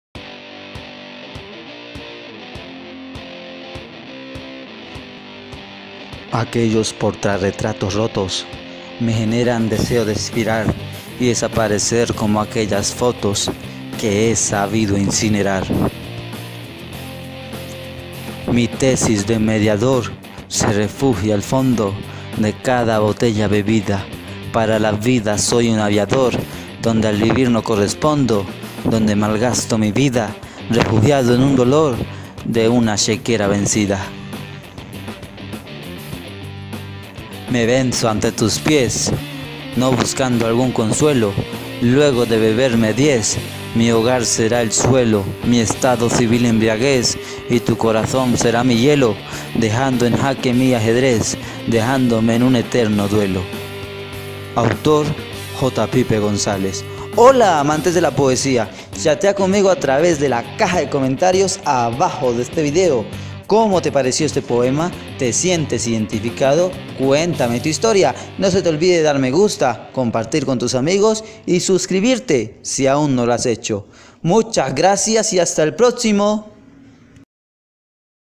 POEMA 6 DE OCTUBRE - UN ETERNO DUELO - DESPECHO - BEBER - DESAMOR - ALCOHOL
Si te ha ido mal en el amor y te refugias en el alcohol, ¡este! es tu poema.